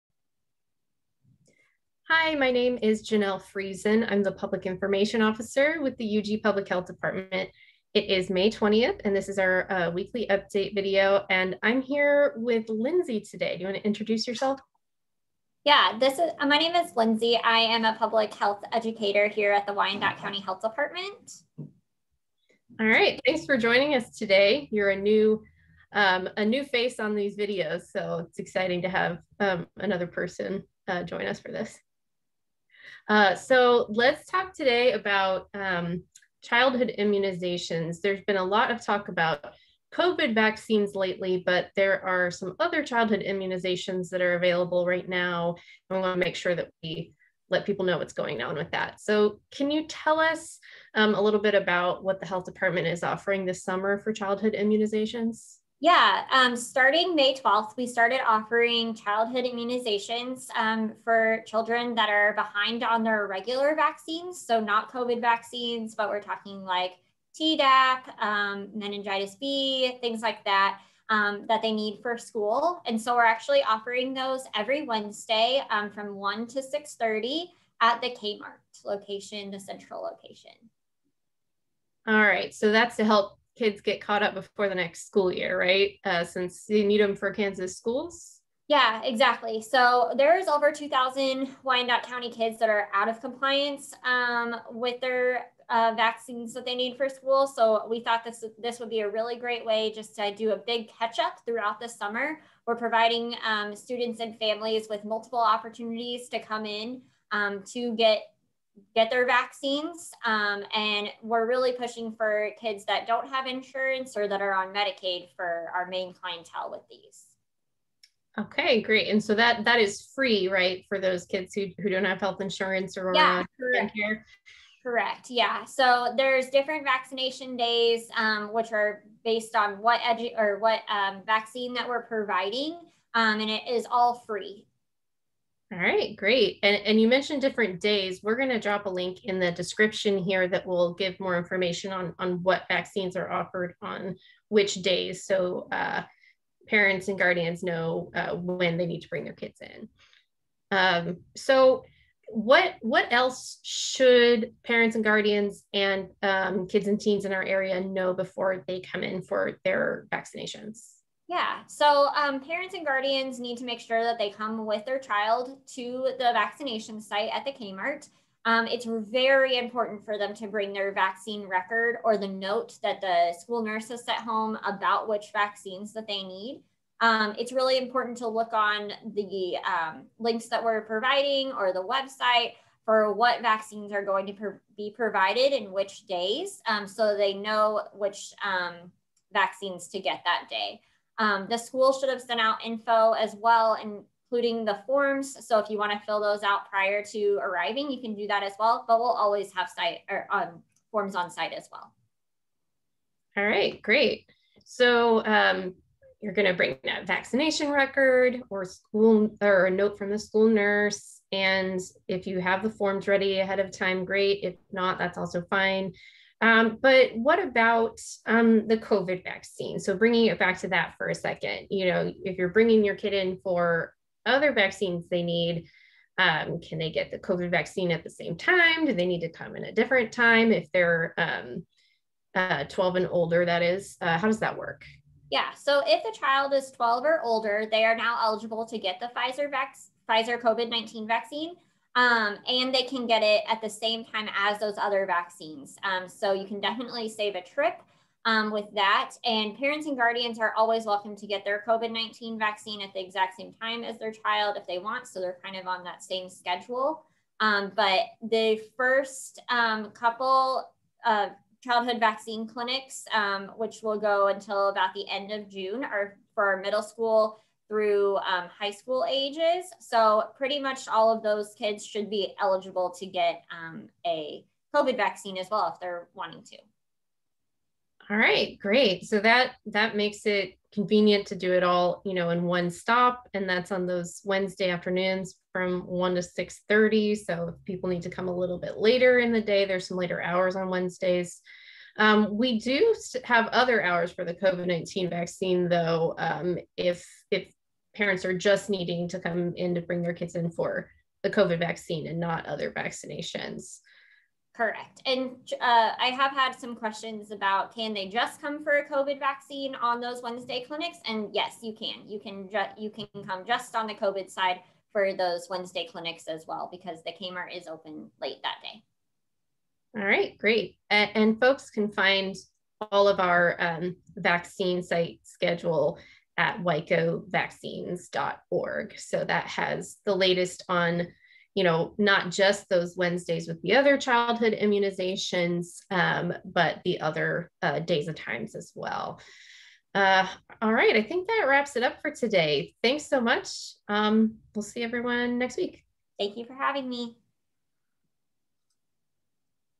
Health Dept. Update: Childhood Immunizations - May 20, 2021
On Wednesdays this summer at 7836 State Ave in KCK, the Unified Government Public Health Department is offering free childhood immunizations (such as TDaP, MMR, etc.) for children who do not have health insurance or who are insured through KanCare (Medicaid). Children age 12+ have the option to receive a COVID-19 vaccine at the same time.